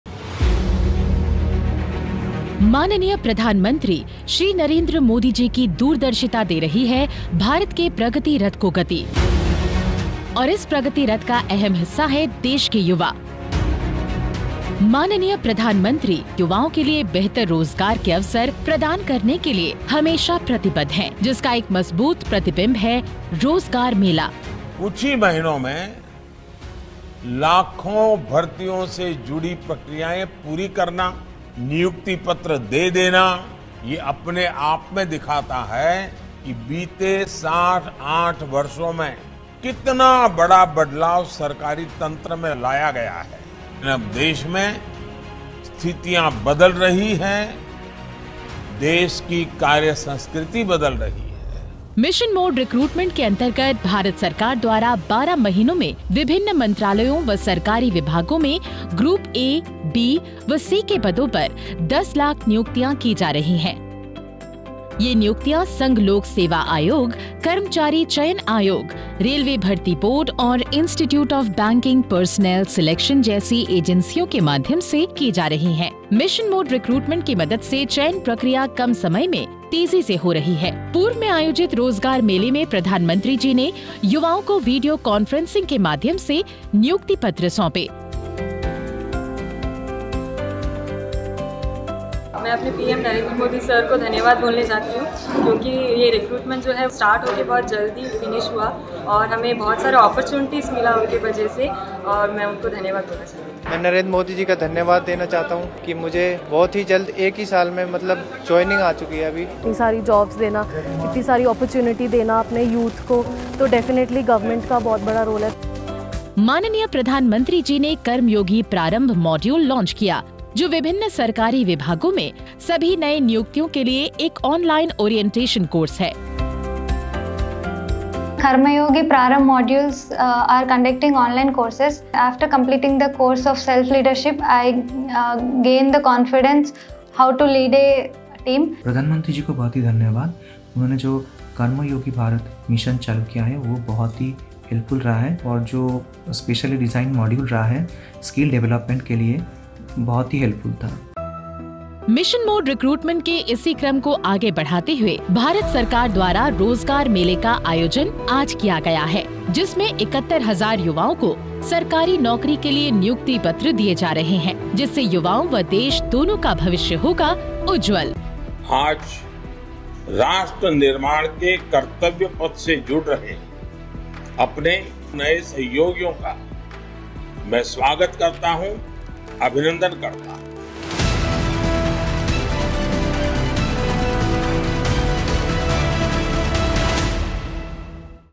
Rozgar Mela – Prioritising employment generation for the youth
Prime Minister Narendra Modi distributed about 71,000 appointment letters to newly inducted recruits in Government departments and organisations, on 20th January, 2023.

The Rozgar Mela is a step towards fulfilment of the commitment of the Prime Minister to accord highest priority to employment generation. The Rozgar Mela is expected to act as a catalyst in further employment generation and provide meaningful opportunities to the youth for their empowerment and participation in national development.

The new recruits, selected from across the country, will join various positions/posts under Government of India like Junior Engineers, loco pilots, Technicians, Inspector, Sub Inspectors, Constable, stenographer, Junior Accountant, Grameen Dak Sevak, Income Tax Inspector, Teacher, Nurse, Doctor, Social security Officer, PA, MTS,  among others.

The experience of newly inducted officials in learning from the Karmayogi Prarambh module, will also be shared during this Rozgar the programme. Karmayogi Prarambh module is an online orientation course for all new appointees in various Government departments.